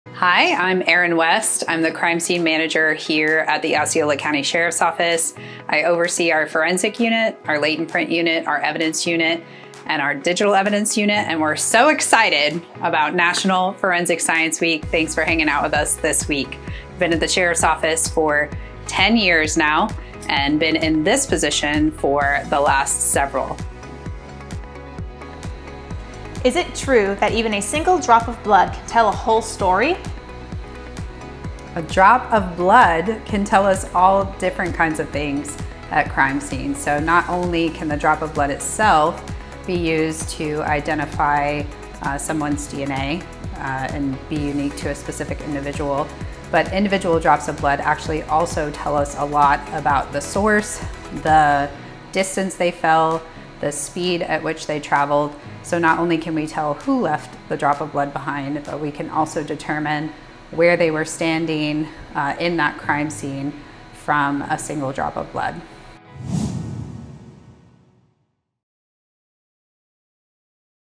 National Forensic Science Week 2025
🎥 National Forensic Science Week 🔬✨
Behind every case… there’s science. 🧪
Behind every answer… there’s precision. 🔍
Behind every investigation… there’s our Forensic Science Unit. 👩‍🔬👨‍🔬
Sheriff Christopher A. Blackmon and the Osceola County Sheriff’s Office thank our forensic experts for their dedication and commitment to justice.